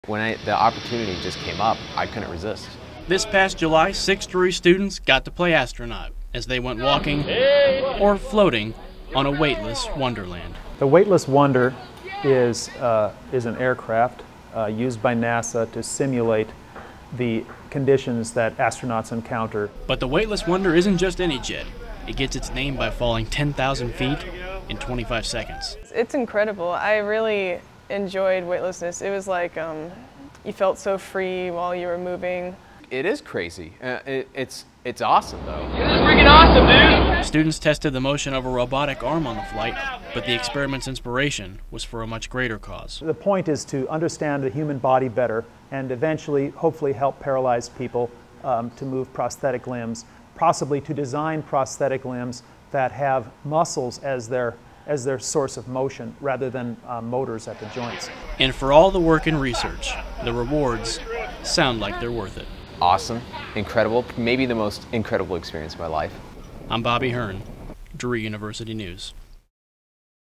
Physics Students Go Weightless with NASA
Drury University's Physics Department was selected as one of only 14 teams nationwide to participate in NASA's 2011 Grant Us Space Reduced Gravity Education Flight Program. As participants in this program, Drury students were given the opportunity to conduct an experiment while flying aboard NASA's Weightless Wonder aircraft. During this experiment, the plane simulates 25-second windows of weightlessness while plummeting over the Gulf of Mexico between 25,000 and 35,000 feet of altitude.